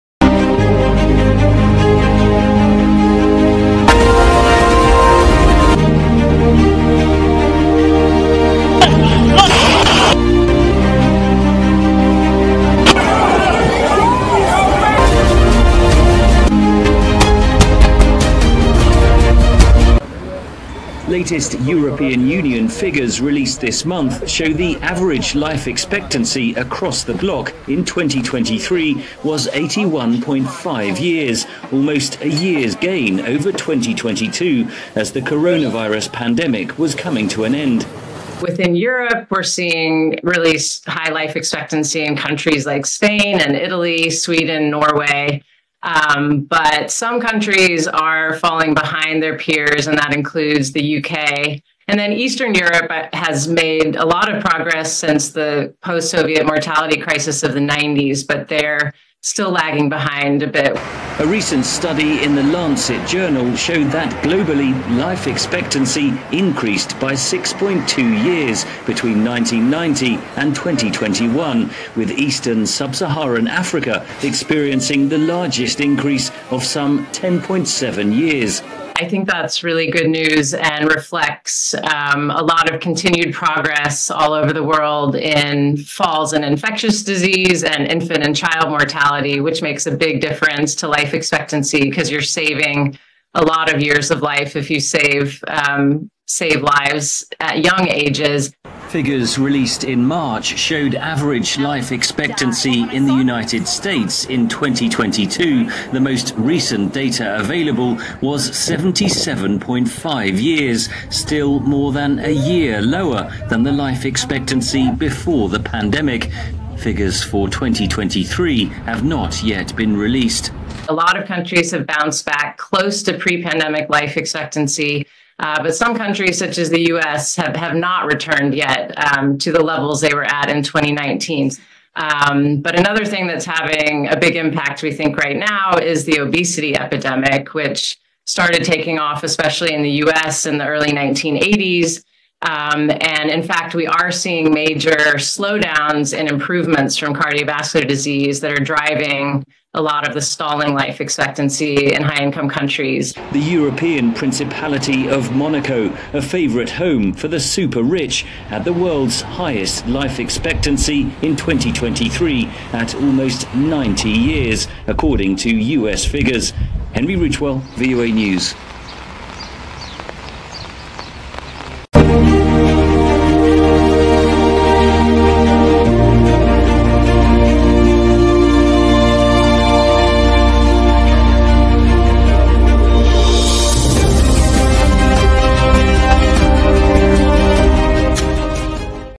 After COVID Pandemic,Life Expectancy Bouncing Back Globally
While the U.S. is still trying to regain lost ground,life expectancy in Europe has returned to the level it was before the 2020 coronavirus pandemic According to recent data, life expectancy has increased in most parts of the world over the past three decades, with the largest increases seen in eastern sub-Saharan Africa.Henry Ridgwell has the report-VOA News